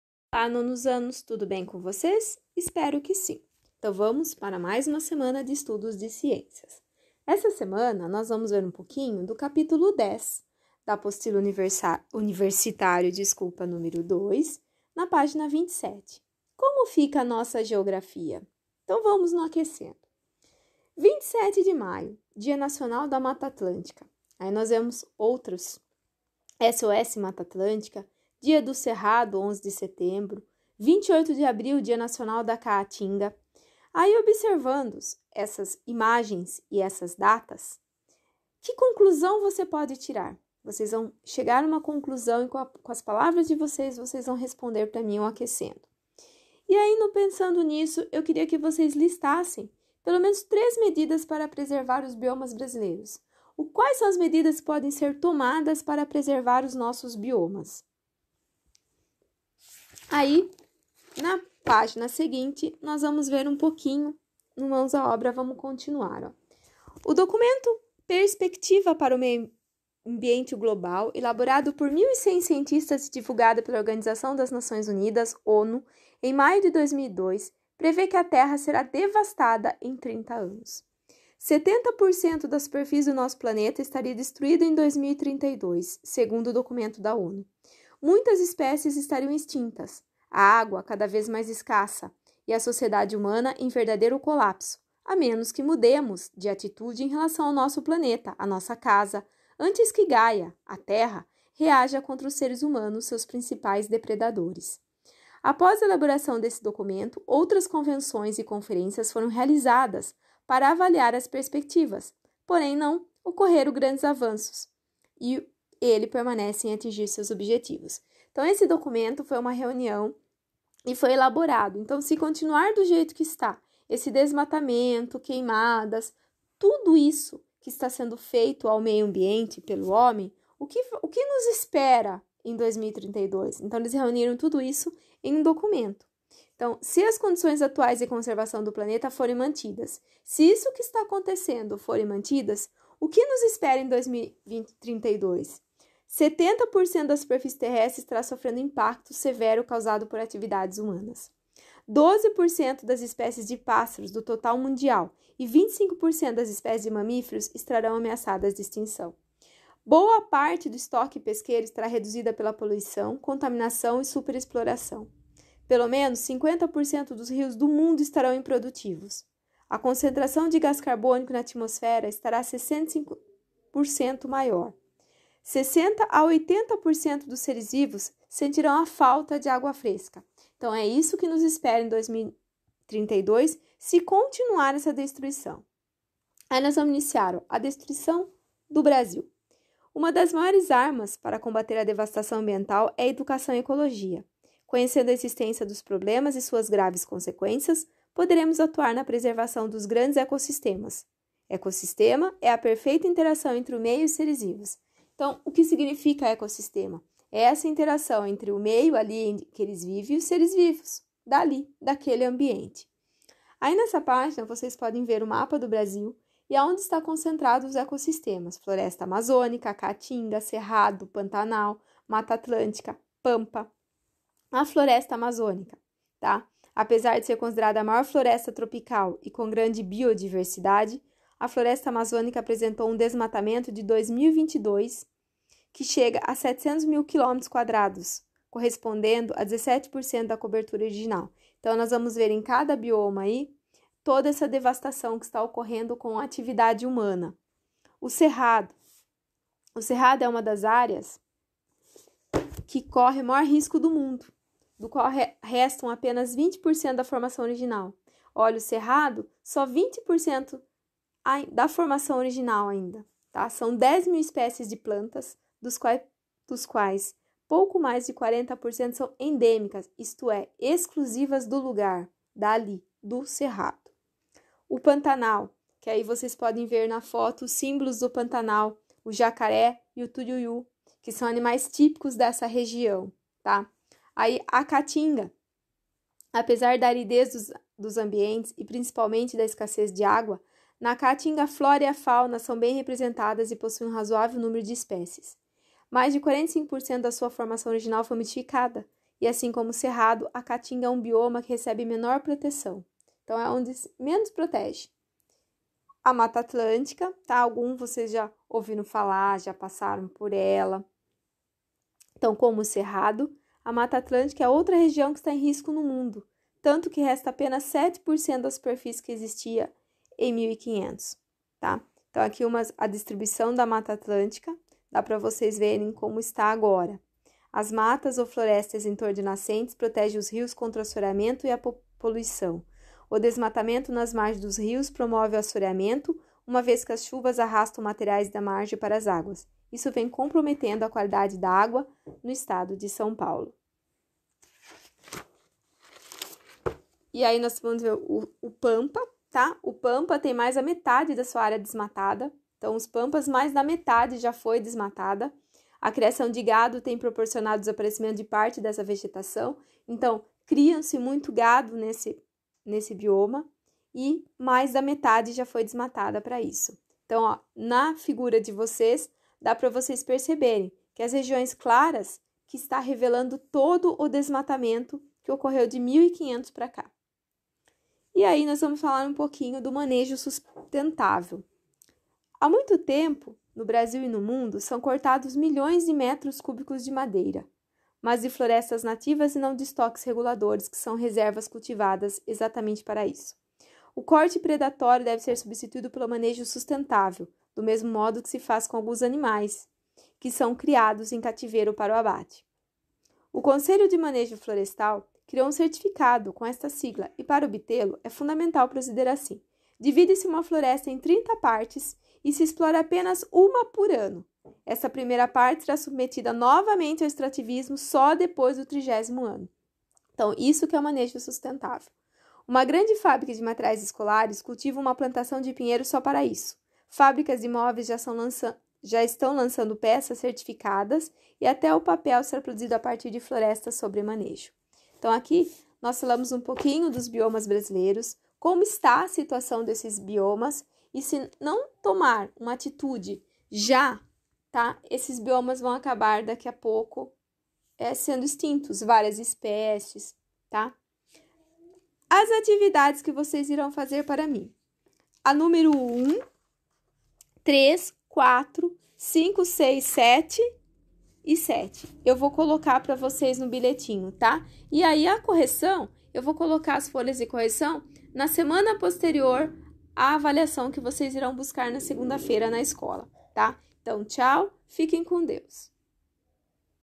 Ciências 9° ano Como fica a nossa Geografia, págs 27 à 34
Bom dia, 9 Ano!
✍ Atividades  de Ciências,  Apostila  Universitário  - Livro 2, capítulo 10, Como fica a nossa Geografia...
👨‍💻 Atividades  para realizar: Aquecendo e Pensando Nisso, página 27 e as questões:1, 3, 4, 5, 6, 7.